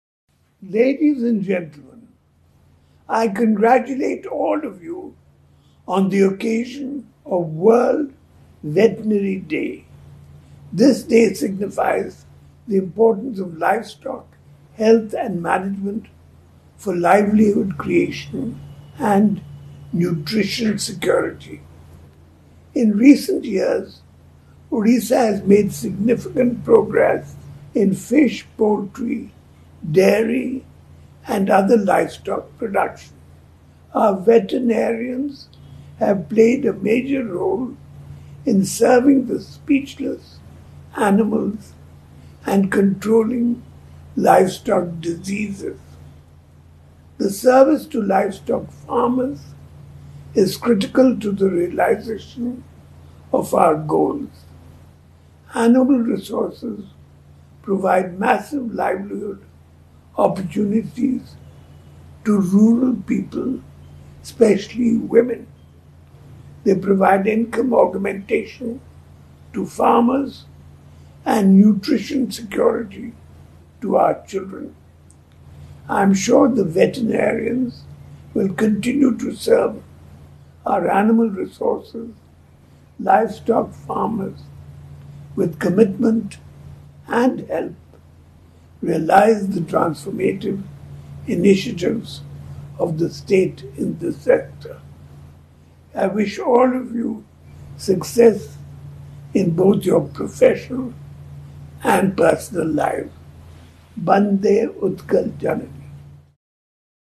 Odisha CM Naveen Patnaik says World veterinary day
Naveen Patnaik development in  odisha // Biju Swasthya Kalyan Yojana scheme helps odisha people

On the occasion of WorldVeterinaryDay, CM @Naveen_Odisha said that this day signifies importance of livestock, health & management for livelihood creation and nutrition security. CM added that Odisha has made significant progress in fish poultry, dairy and livestock production.